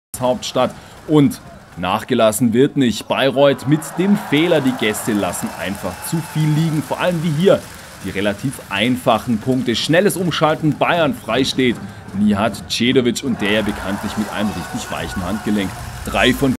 Nihad Djedovic: Amazing defence and offence skills
Nihad Djedovic: Amazing defence and offence skills vs Medi Bayreuth 103-70